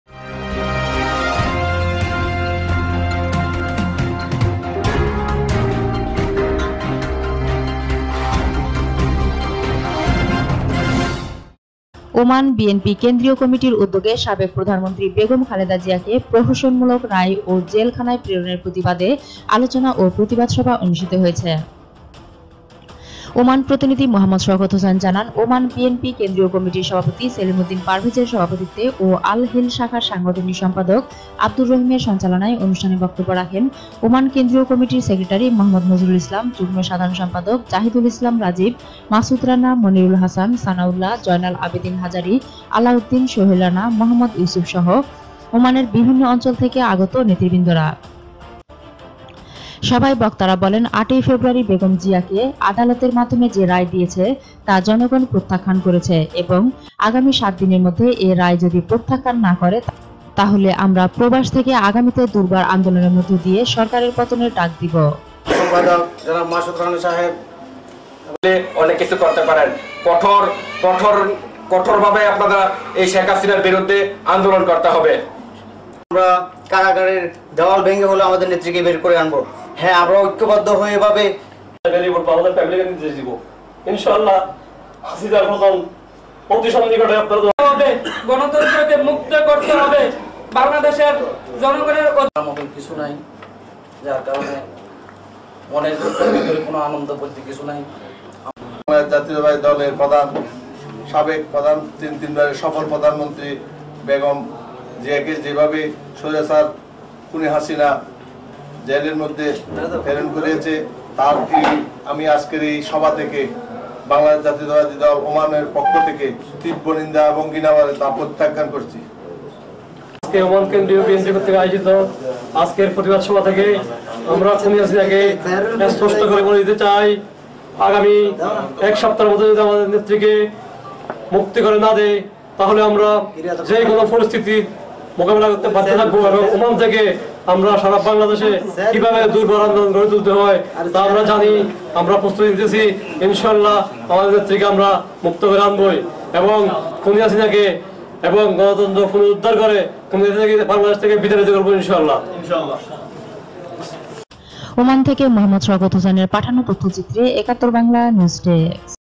সাবেক প্রধান মন্ত্রী খালেদা জিয়াকে প্রহসন মূলক রায় ও জেল খানায় প্রেরনে প্রতিবাদ সভা|Oman|71Bangla TV
ওমান বি,এন,পি, কেন্দ্রিয় কমিটির উদ্দ্যেগে সাবেক প্রধান মন্ত্রী বেগম খালেদা জিয়াকে  প্রহসন মূলক রায় ও জেল খানায় প্রেরনের প্রতিবাদে আলোচনা ও প্রতিবাদ সভা অনুষ্টিত হয়েছে।
ওমান প্রতিনিধি মোহাম্মদ শওকত হোসাইন জানান, ওমান বি,এন,পি কেন্দ্রিয় কমিটির সভাপতি সেলিম উদ্দিন পারভেজের সভাপতিত্বে ও আল হিল শাখার সাংগঠনিক সম্পাদক আব্দুর রহিমের সঞ্চালনায় অনষ্টানে বক্তব্য রাখেন, ওমান কেন্দ্রিয় কমিটির সেক্রেটারী মোহাম্মদ নজরুল ইসলাম, যুগ্ম সাধারন সম্পাদক জাহিদুল ইসলাম রাজিব,মাসুদ রানা, মনিরুল হাসান,সানাউল্লাহ, জয়নাল আবদীন হাজারী, আলাউদ্দিন, সোহেল রানা, মোহাম্মদ ইউচুপ সহ ওমানের ভিবিন্ন অঞ্চল থেকে আগত নেতৃবৃন্দরা।
সভায় বক্তরা বলেন, ৮ই ফেব্রুয়ারী বেগম জিয়ারকে আদালতের মাধ্যমে যে রায় দিয়েছে তা জনগন প্রত্যাখ্যান করেছে, এবং আগামী ৭ দিনের মধ্যে এই রায় যদি প্রত্যাখ্যান না করে তাহলে আমরা প্রবস ধেকে আগামীতে দুর্বার আন্দোলনের মধ্য দিয়ে সরকার পতনের ডাক দিব।